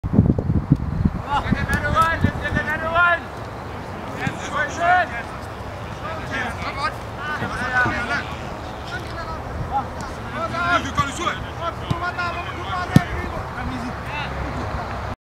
This Is How Pro's Move The ball Part 2
Some video snippets of a friendly between FC Free Agents and Supersport United at MegaWatt Park on December 2nd.